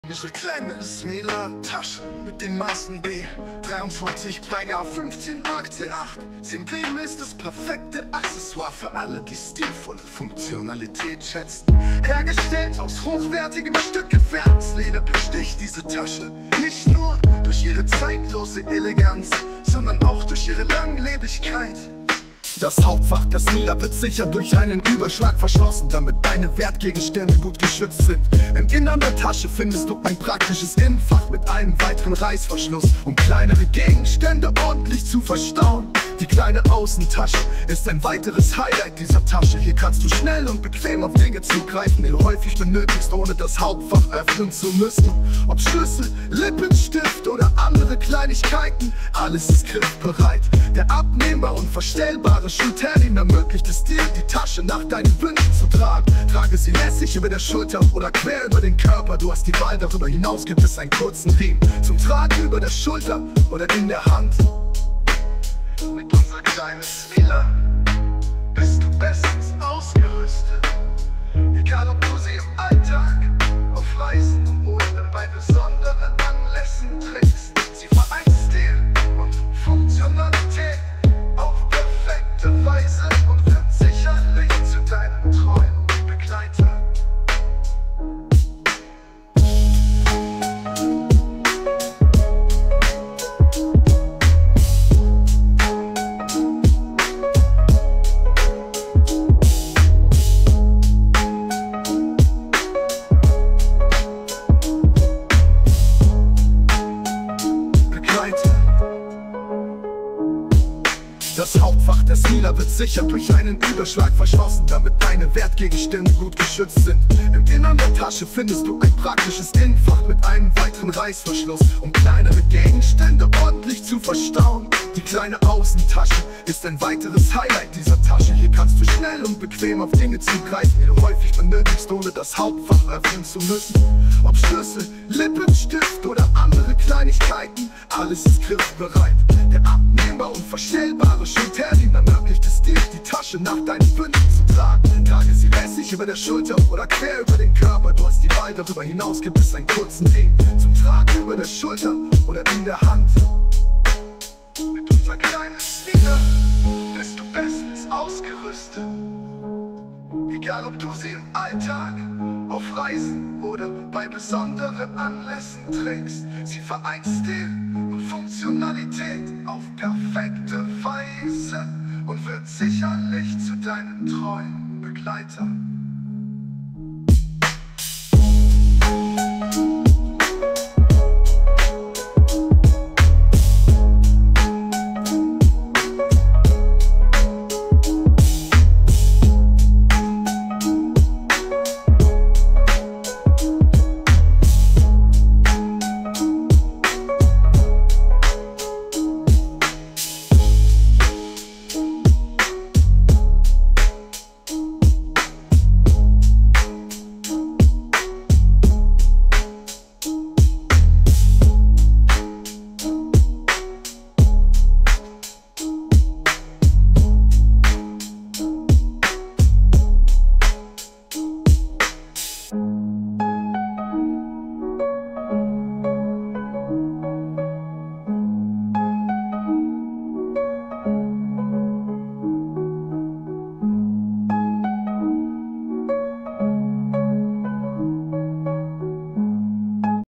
SMILA LIGHT TAN - BULL&HUNT - die erste
BULL&HUNT
BE SMART - BE NATURAL - BE SUSTAINABLE

Unsere kleine Smila Tasche mit den Maßen B 23 x H 15 x T 8 cm ist das perfekte Accessoire für alle, die stilvolle Funktionalität schätzen. Hergestellt aus hochwertigem, stückgefärbtem Rindsleder, besticht diese Tasche nicht nur durch ihre zeitlose Eleganz, sondern auch durch ihre Langlebigkeit.

Das Hauptfach der Smila wird sicher durch einen Überschlag verschlossen, damit deine Wertgegenstände gut geschützt sind. Im Inneren der Tasche findest du ein praktisches Innenfach mit einem weiteren Reißverschluss, um kleinere Gegenstände ordentlich zu verstauen.

Die kleine Außentasche ist ein weiteres Highlight dieser Tasche. Hier kannst du schnell und bequem auf Dinge zugreifen, die du häufig benötigst, ohne das Hauptfach öffnen zu müssen. Ob Schlüssel, Lippenstift oder andere Kleinigkeiten – alles ist griffbereit.

Der abnehmbare und verstellbare Schulterriemen ermöglicht es dir, die Tasche nach deinen Wünschen zu tragen. Trage sie lässig über der Schulter oder quer über den Körper – du hast die Wahl. Darüber hinaus gibt es einen kurzen Riemen zum Tragen über der Schulter oder in der Hand.

Mit unserer kleinen Smila bist du bestens ausgerüstet, egal ob du sie im Alltag, auf Reisen oder bei besonderen Anlässen trägst. Sie vereint Stil und Funktionalität auf perfekte Weise und wird sicherlich zu deinem treuen Begleiter.

BULL&HUNT zip&friends GmbH & Co. KG
Otto-Volger-Str. 9b
65843 Sulzbach

Vertreten durch:
Matthias Altfeld